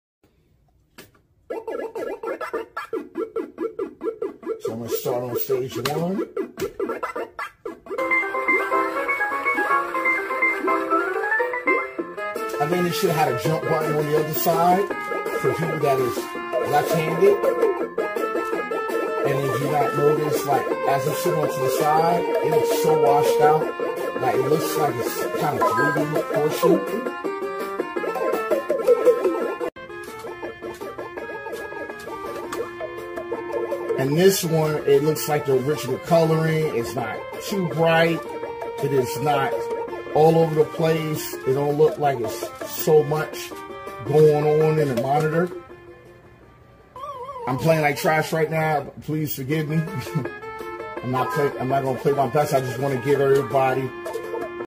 Arcade1up "TN" vs "BOE" Monitor - What's the difference?
The difference between the Arcade1up Pacmania “TN” (Twisted Nematic) Monitor and “BOE” Monitor primarily lies in their display technologies and performance characteristics:

Contrast Ratio: The TN monitor mentioned in the context of the Arcade1up Pacmania has a contrast ratio of 1000:1, which is about twice as high as the BOE monitors. This means that the colors on the TN monitor will appear more vibrant and pop more compared to the BOE monitors1.

Response Rate: The TN monitor also has a faster response rate than the BOE monitors, which can be a significant advantage for gaming as it reduces motion blur and provides a smoother experience1.

Viewing Angles: TN monitors generally have narrower viewing angles compared to BOE monitors, which use IPS (In-Plane Switching) technology. However, the specific TN monitor in question seems to perform better than the stock monitor, offering more viewable and playable angles. From most angles, the display looks vibrant and colorful, although there is some color shift from extreme angles1.

Design and Compatibility: Discussions on forums suggest that different Arcade1up cabinets have been equipped with either TN or BOE monitors, with the newer generations tending to have BOE IPS panels. The choice between TN and BOE may also affect the physical design aspects of the cabinet, such as screen tilt and control panel layout2.

In summary, the TN monitor offers better contrast and response time, which are crucial for gaming, while the BOE monitors might offer better viewing angles and color accuracy. The choice between the two could depend on personal preference and the specific gaming experience one is looking for.